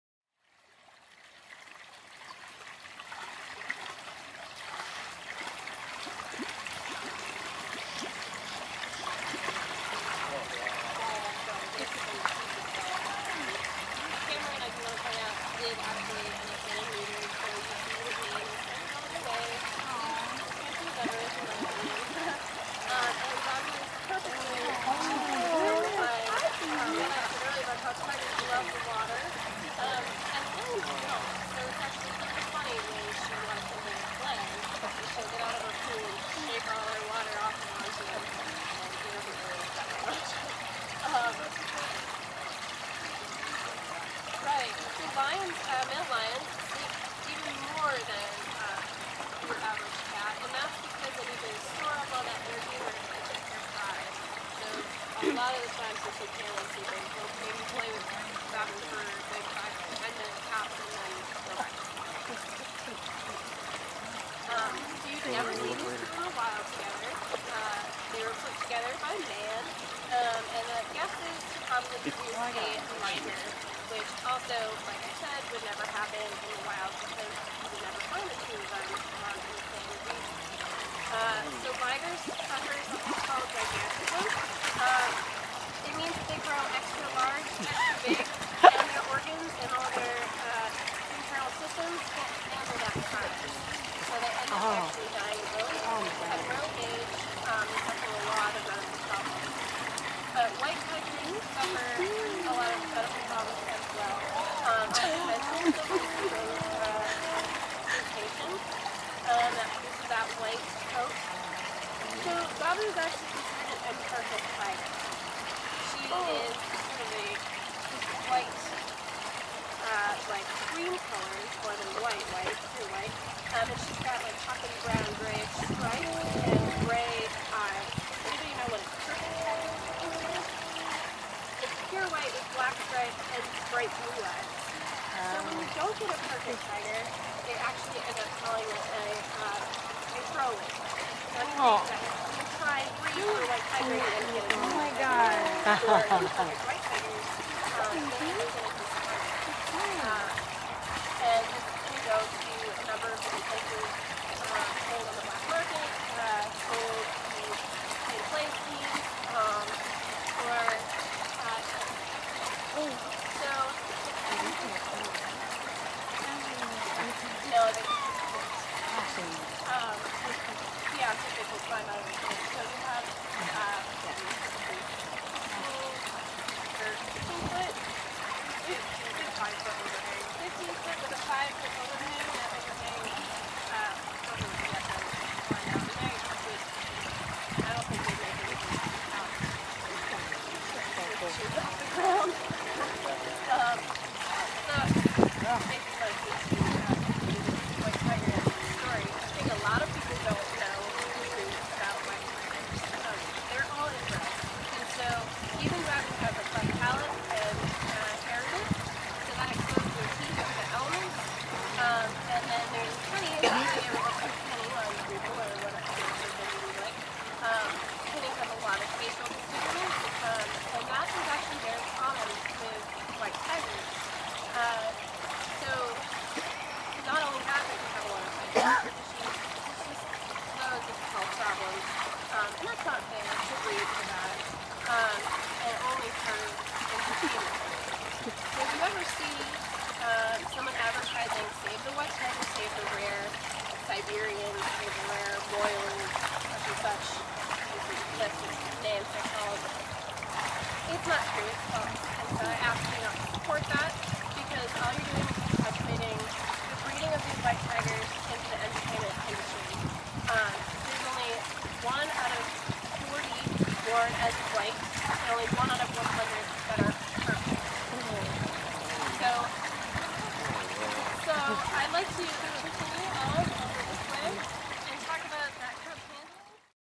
White Tiger
White Tiger in captivity at Big Cat Rescue, Tampa Florida. This tiger is mated with a male Lion to produce Ligers.